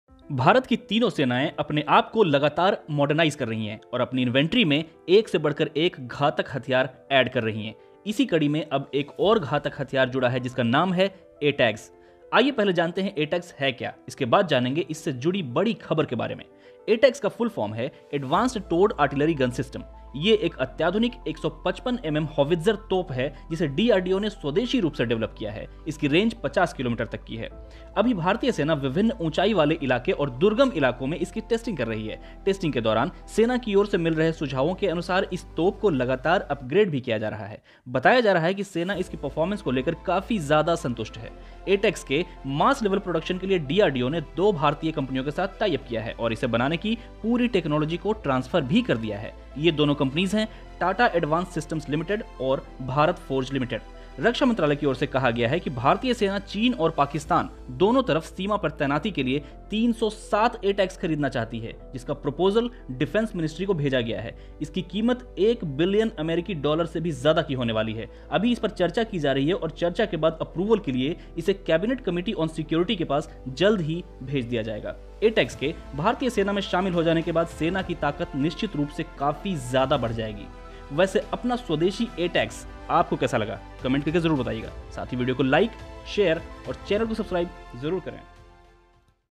स्वदेशी ATAGS पर आई बड़ी खबर । Big Breaking on ATAGS Howitzer ।
भारत की तीनों सेनाएं अपने आप को लगातार मोडर्नाइस कर रही हैं और अपनी इन्वेंट्री में एक से बढ़कर एक घातक हथियार ऐड कर रही हैं।अब इसी कड़ी में एक और घातक हथियार जुड़ा है जिसका नाम है ATAGS।
आइए पहले जानते हैं कि ATAGS है क्या? उसके बाद जानेंगे इससे जुड़ी बड़ी खबर के बारे में।
ATAGS का फूल फॉर्म है एडवांस्ड टोड आर्टिलरी गन सिस्टम। ये एक अत्याधुनिक 155 mm होवित्जर तोप है जिसे DRDO ने स्वदेशी रूप से डेवेलप किया है।